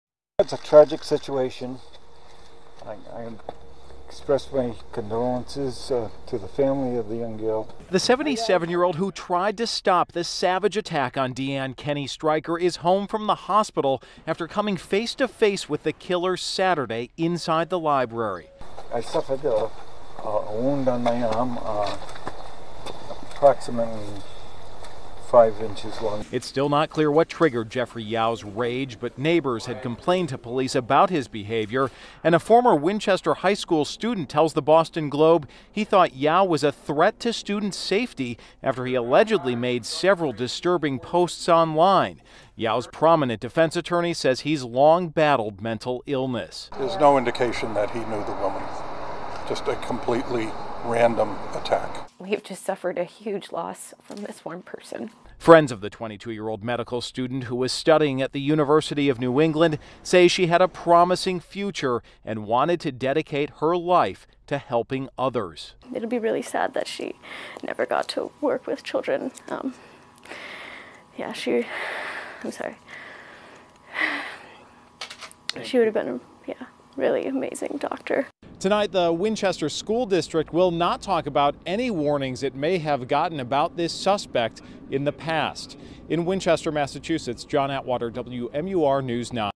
Suspect in Winchester, MA library murder held without bail
A man accused in the fatal stabbing of a medical student who was attacked while studying inside the Winchester library appeared in court Monday.